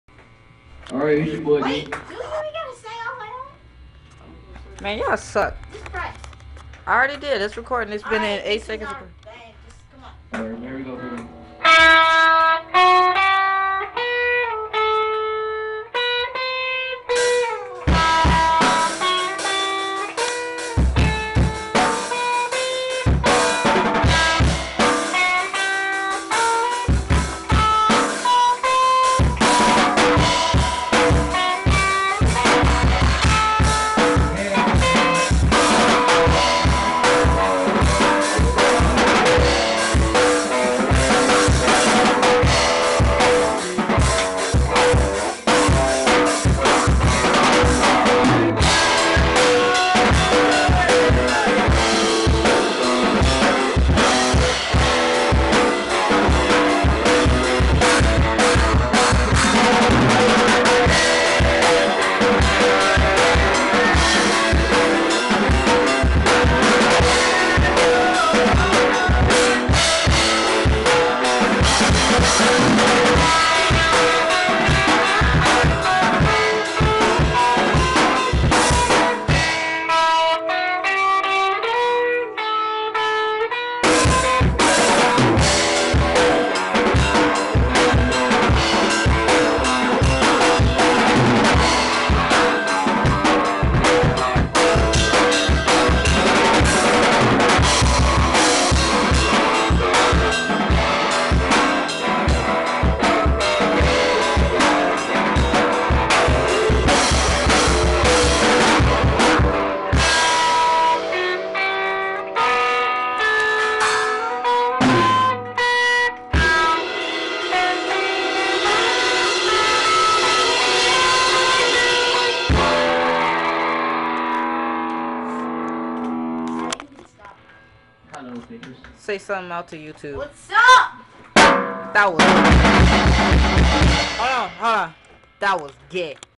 little jam
the guy whos playing guitar dosent no how to listen to drums though but o welland my brother is filming